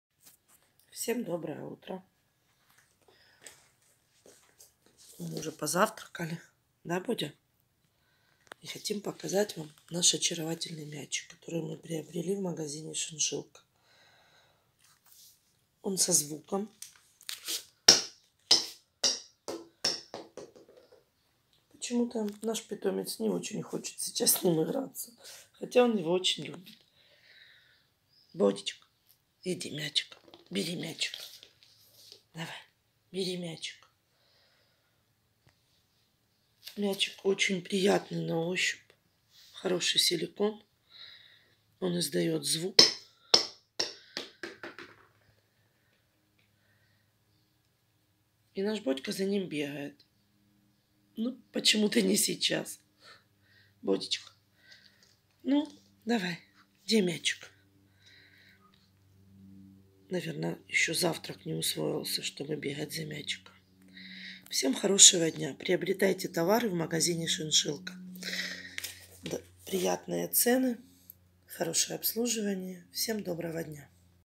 Trixie TX-33678 игрушка,издающая звук при движении
Термопластичный каучук (TPR), прочный и долговечный, со звуком, разных цветов